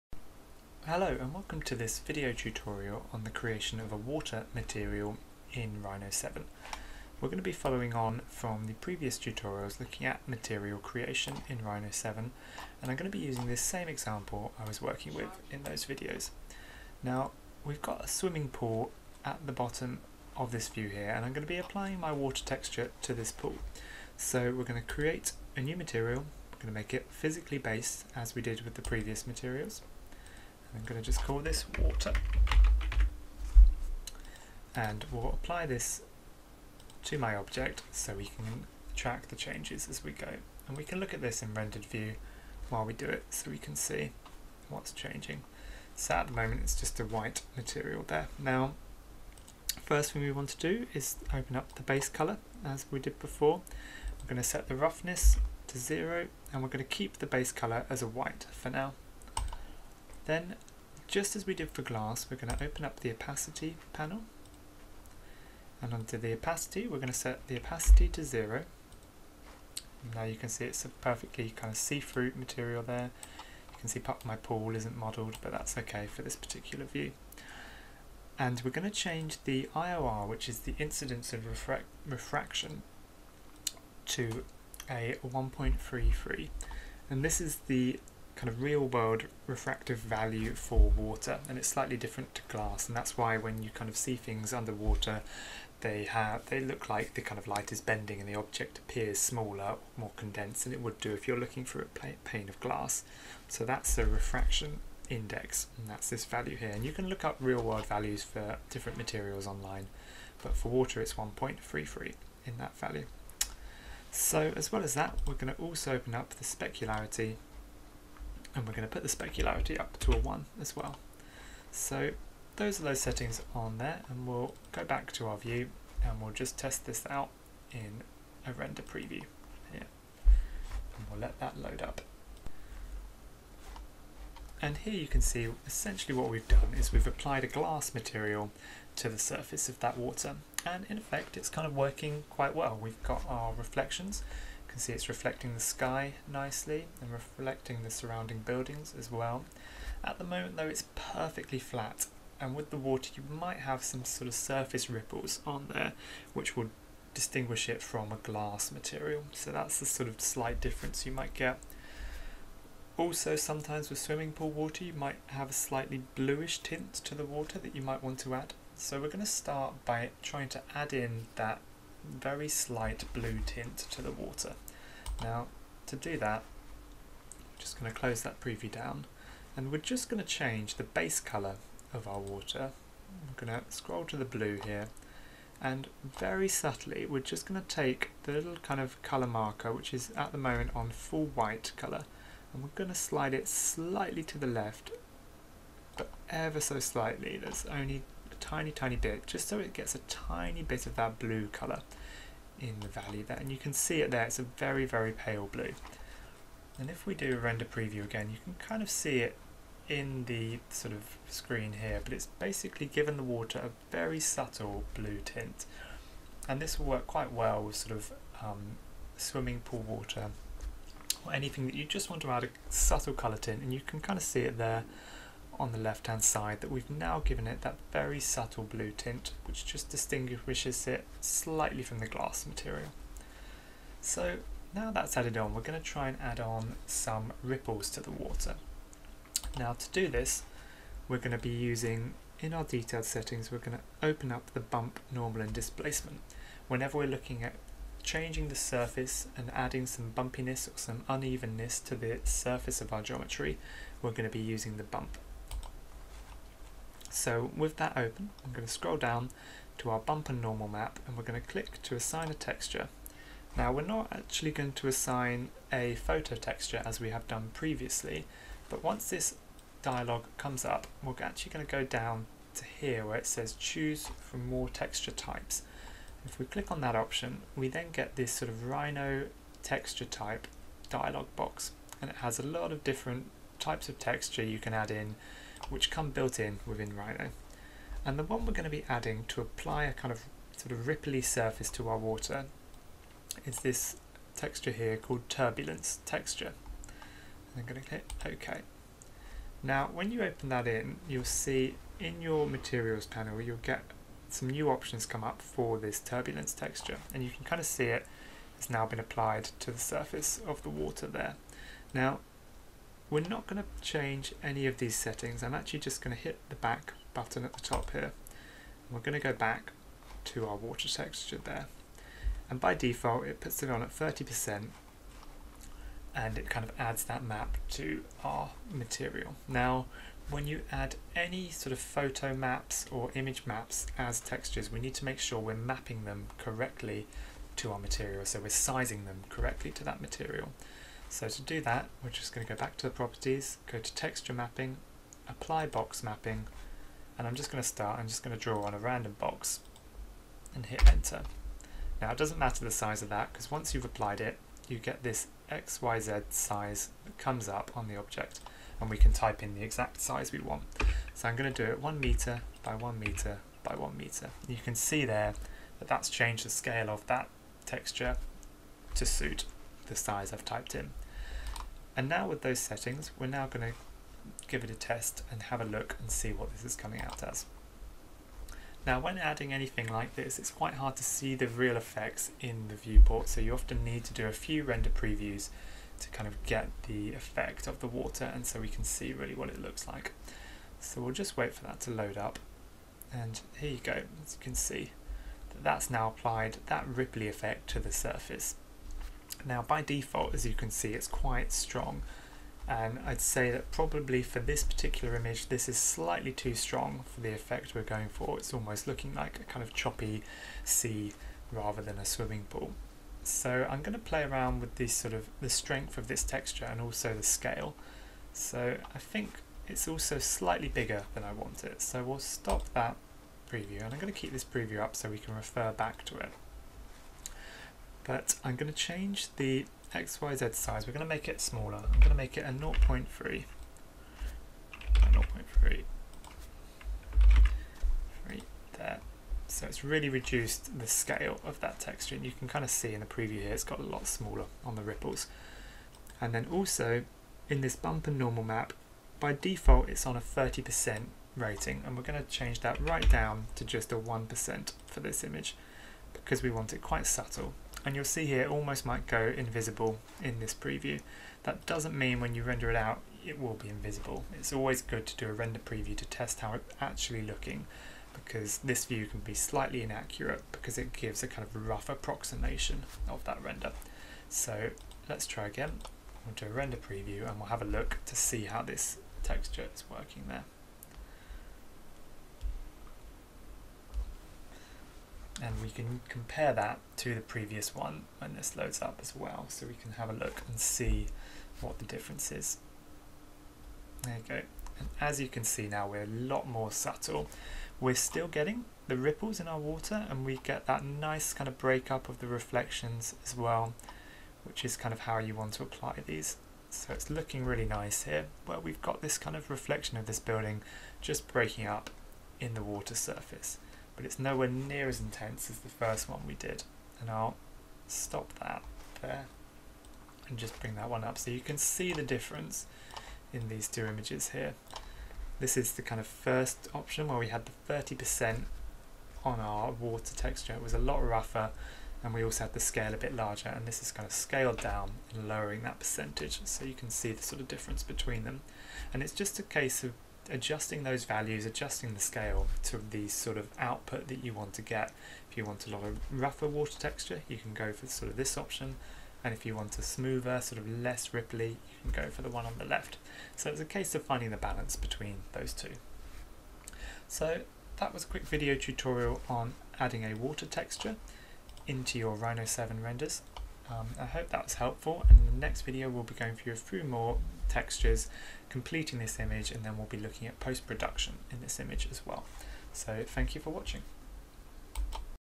Rhino 7 - Water Material Creation - PBR
This video tutorial goes through the steps to create a realistic water material in Rhino 7 using PBR materials. Timestamps to the key sections in this video can be found below:

0:25 - Creating a physically based material
0:50 - Base Colour
0:59 - Opacity and IOR settings
1:53 - Specularity settings
2:05 - Render preview test
2:40 - Adding a blue tint to the water
3:55 - Using a 'Turbulence' Bump map to give ripples to the water
5:30 - Texture Mapping to adjust the scale of the ripples
6:50 - Adjusting the scale and intensity of the water ripples